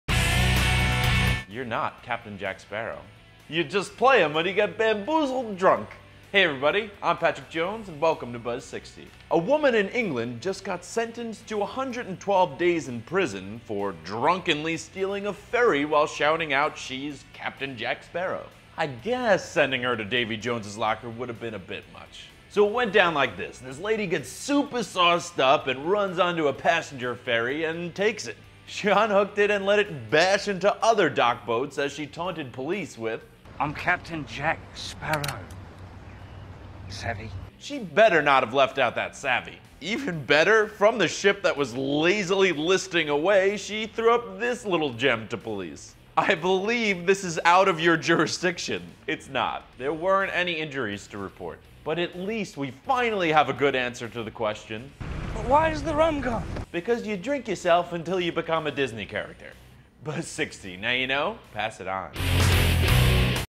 Drunk Woman Steals Ferry Boat, Claims She's Jack Sparrow
A woman has been sentenced to 112 days in prison for hijacking a passenger ferry in England as she screamed out "I'm Jack Sparrow," referencing the Pirates of the Caribbean character. Alison Whelan had a good deal to drink before stealing the boat, which had no passengers aboard, and then smashing it into others as she floated away. Patrick Jones has the details.